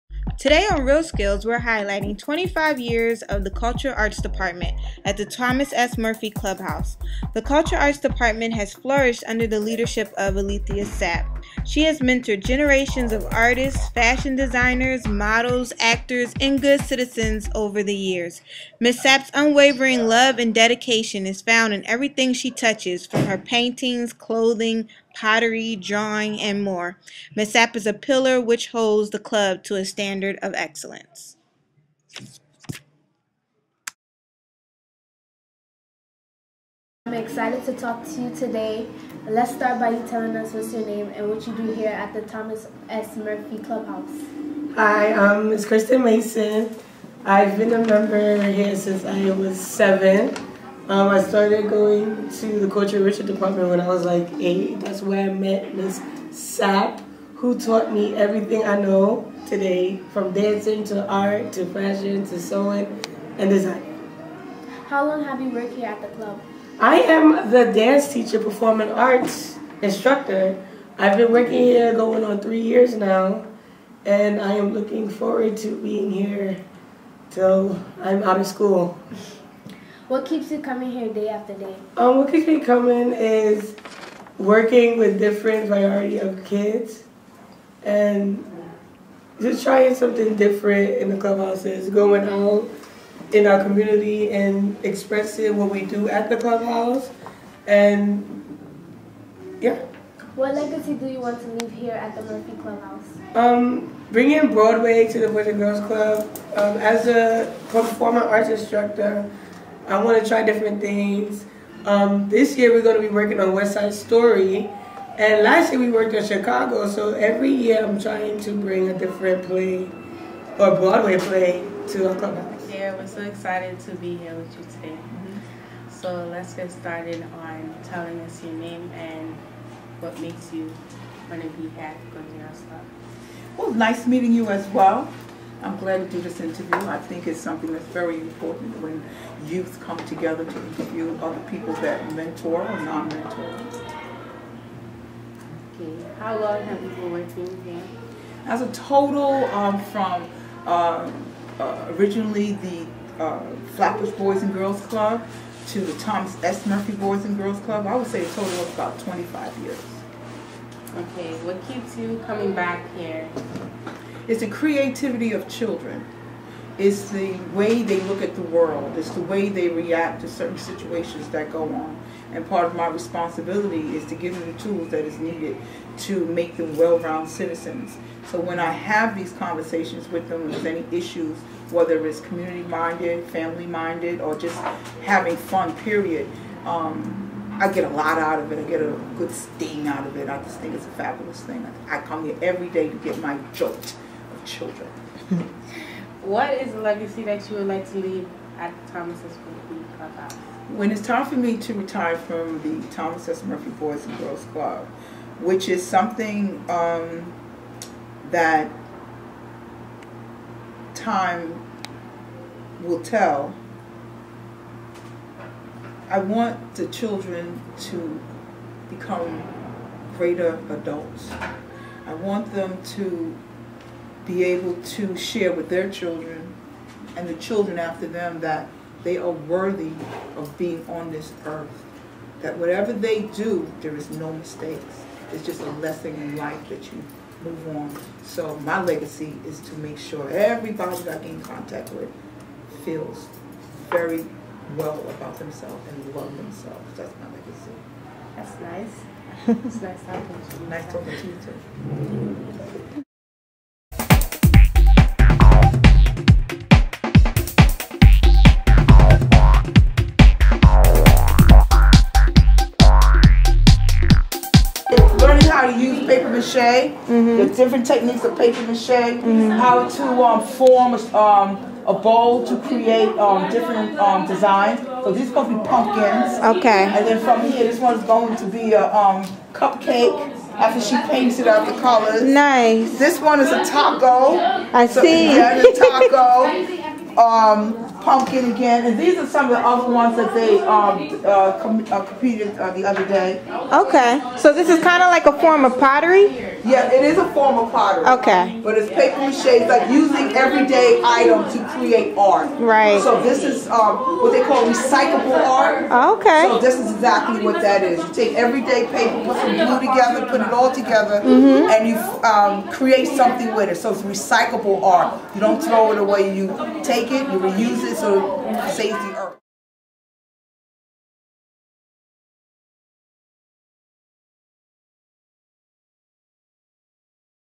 CULTURAL ARTS DEPARTMENT AT THE MURPHY CLUBHOUSE
An inside peek into the cultural arts department at the Thomas S. Murphy Clubhouse in Brooklyn, NY. Film by Reel Skills students.